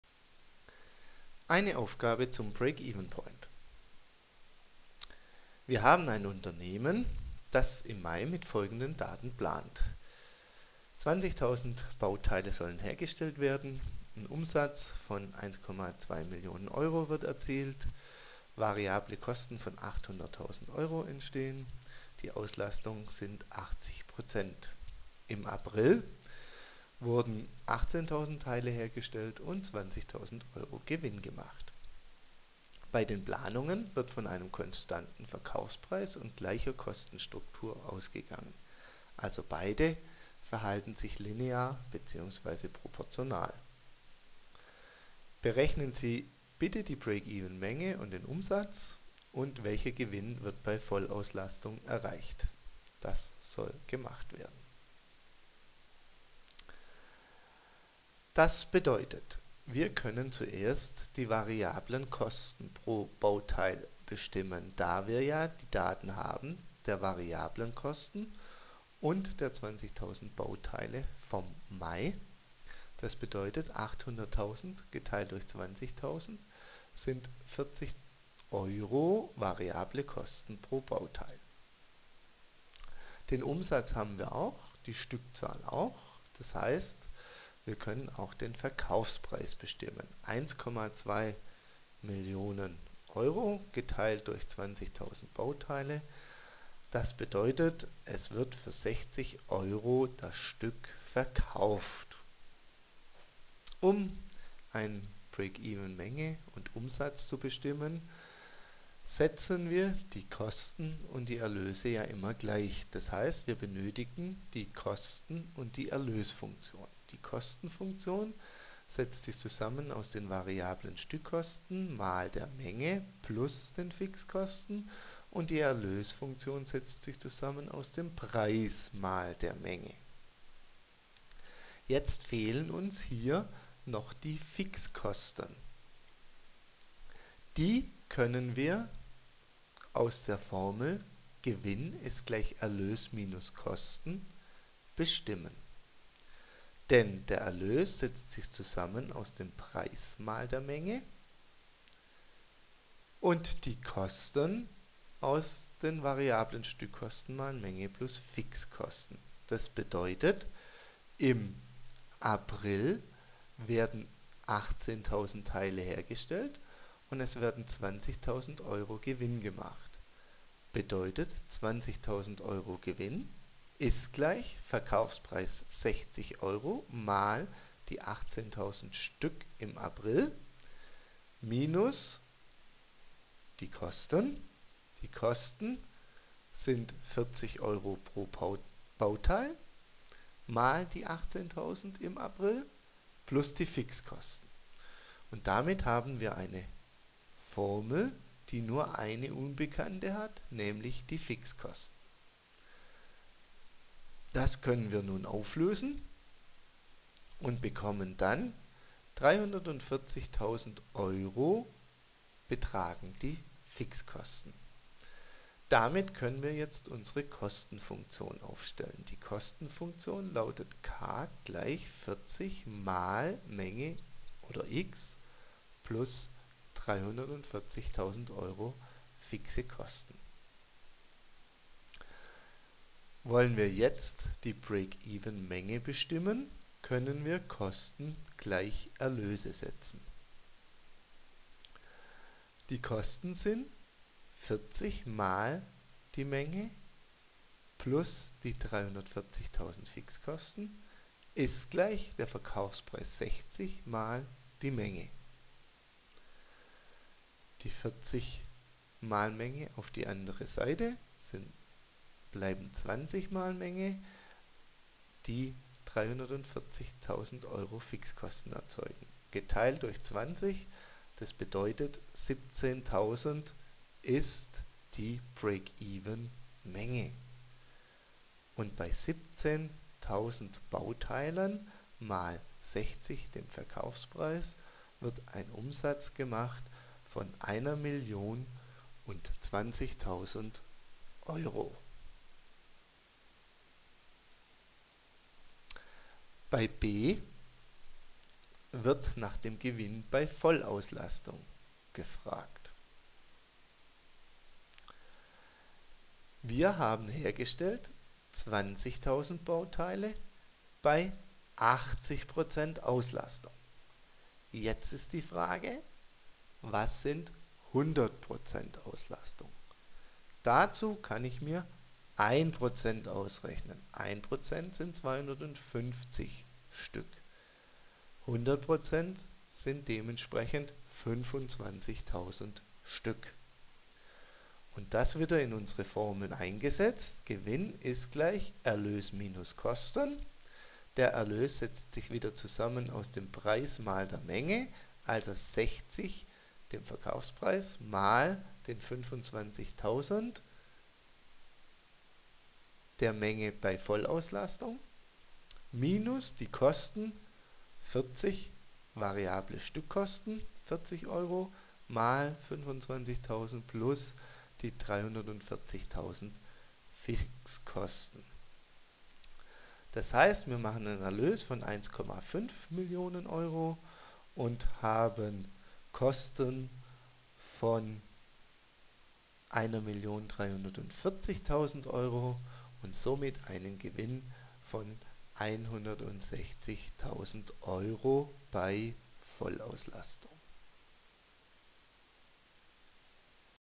Break-even point - Aufgabe
Eine Aufgabe mit Lösung zum Thema Break-even point bzw. Gewinnschwelle